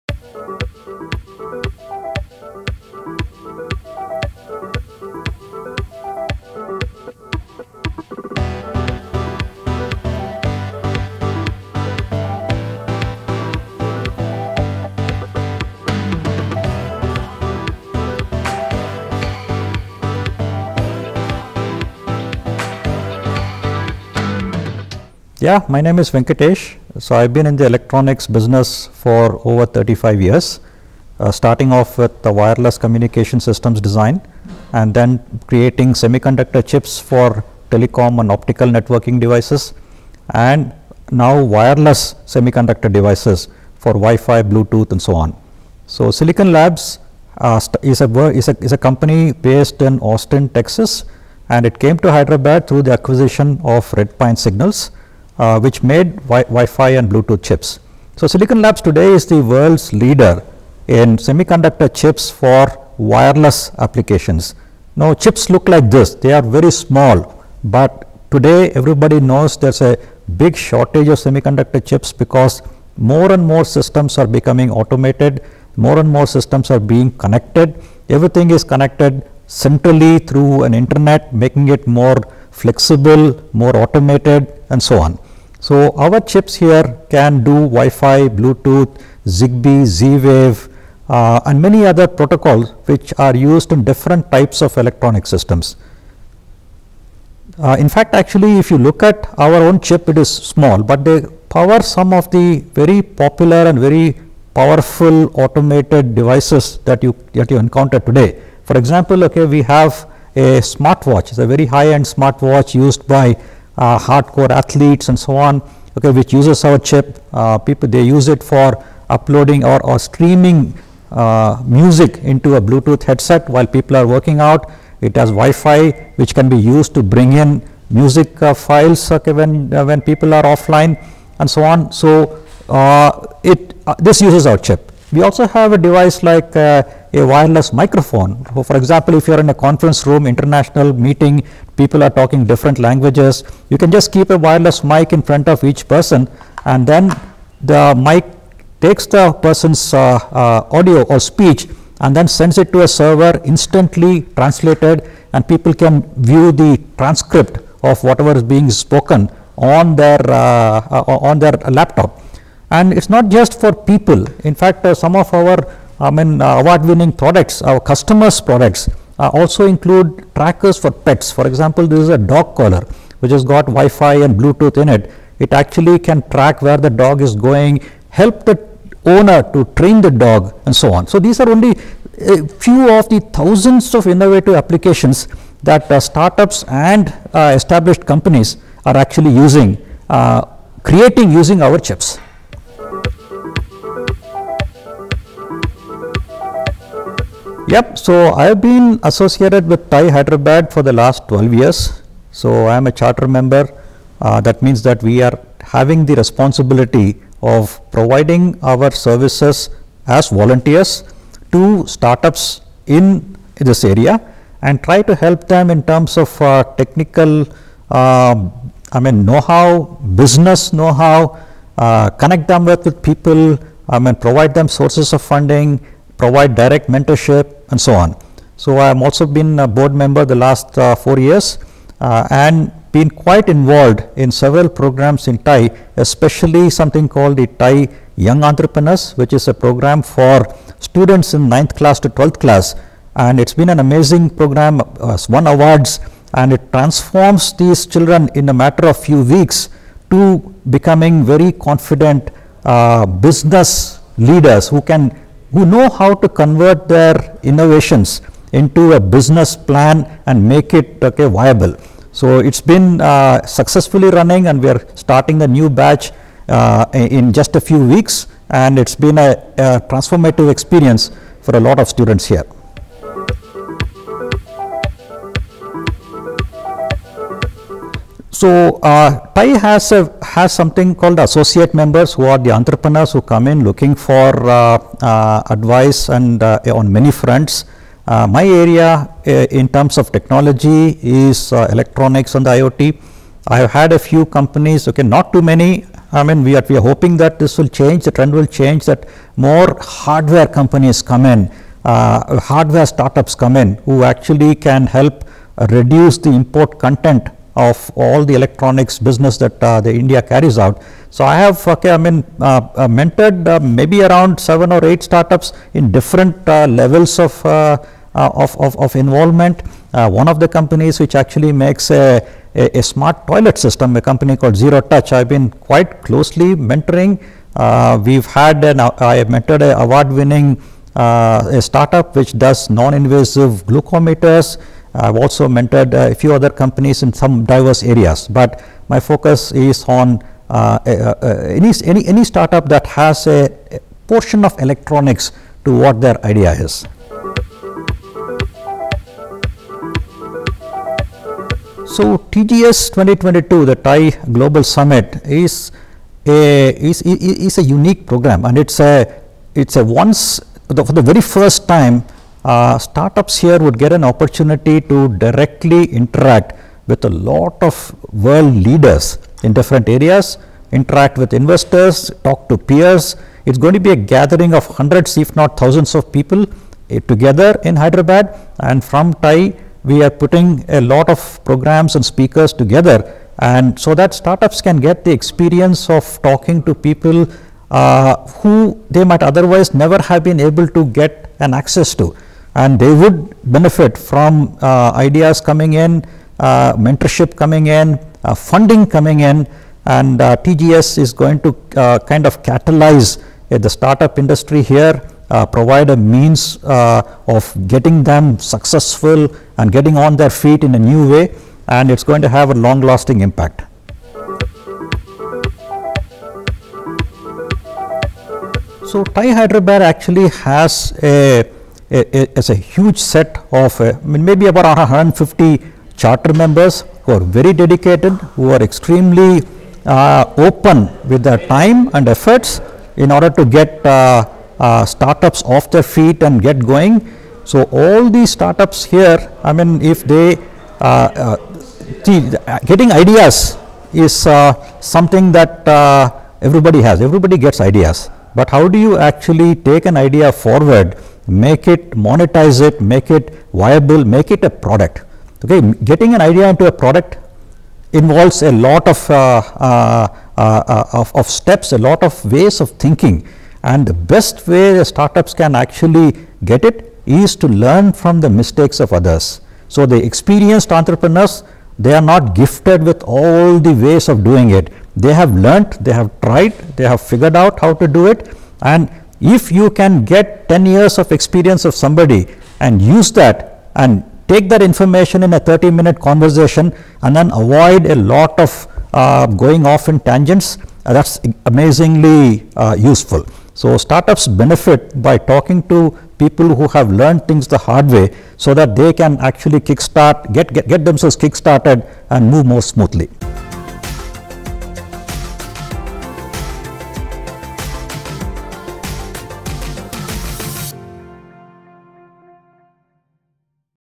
Silicon Labs create Semi Conductor Chips for Telecom | TiE Global Summit 2022 | N.Venkatesh
Interview with Silicon Labs Sr. Director N.Venkatesh | TiE Global Summit 2022